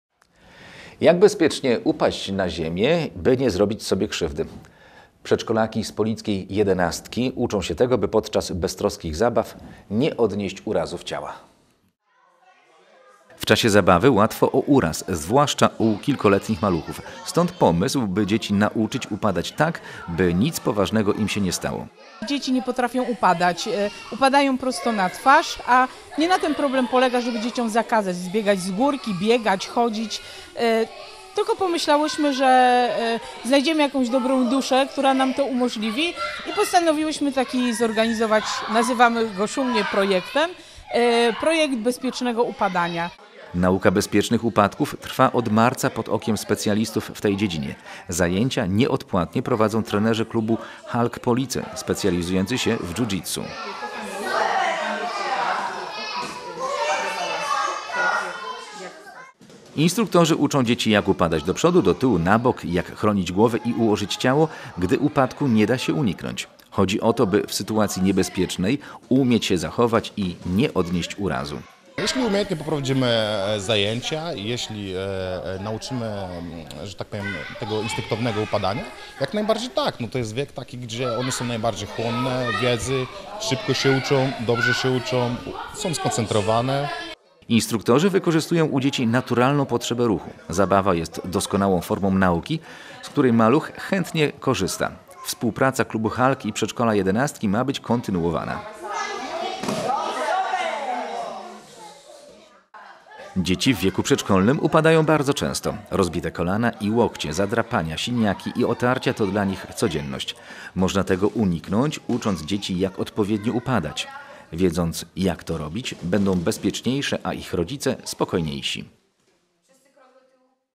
Nauka bezpiecznego upadania
Teletydzień 16.06.17- Jak bezpiecznie upaść na ziemię, by nie nie zrobić sobie krzywdy? Przedszkolaki z polickiej jedenastki uczą się tego, by podczas beztroskich zabaw nie odnieść urazów ciała.